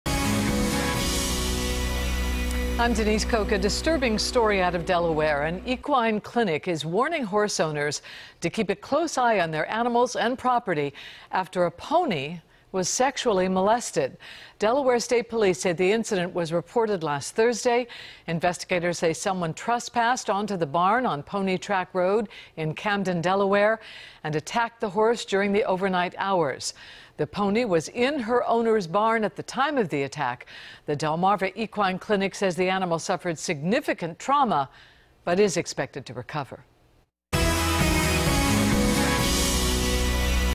Equine Clinic: Pony Sexually Assaulted At Delaware Barn
An equine clinic in Delaware is warning horse owners to keep a close eye on their animals and property after a pony was sexually molested.